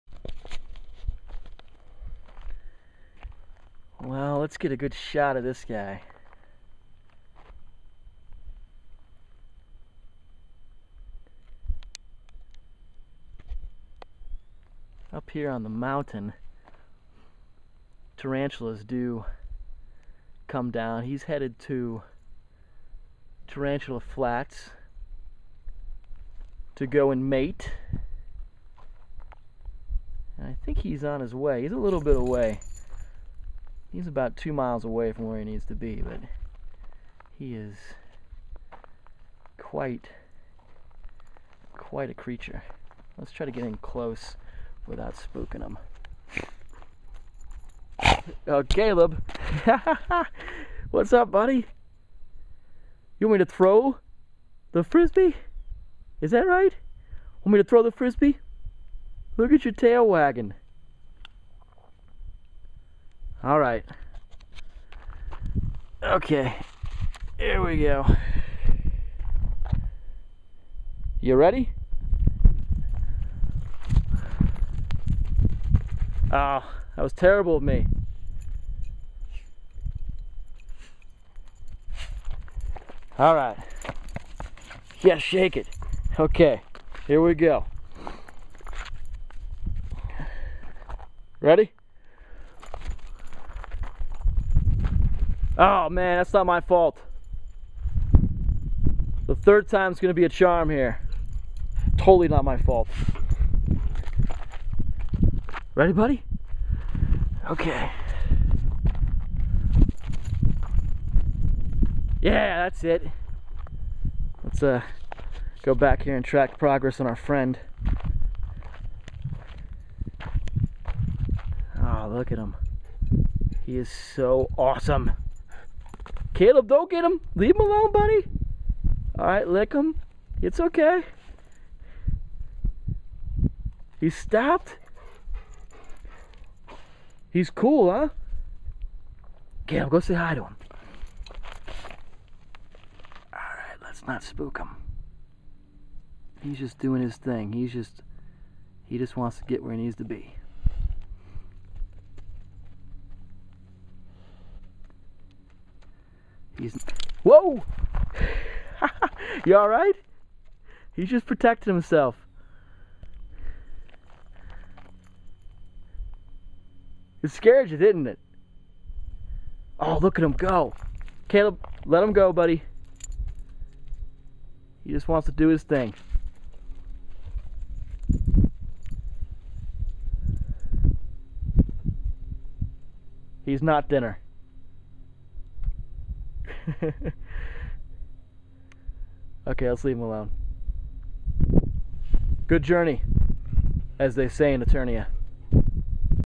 World Adventure: CB, Caleb and the Tarantula!
CB and Caleb the Australian Shepherd encounter a tarantula on his way to the mating grounds of Tarantula Flats. Somehow they manage to get a couple of frisbee throws in before Caleb decides the spider might make a good snack!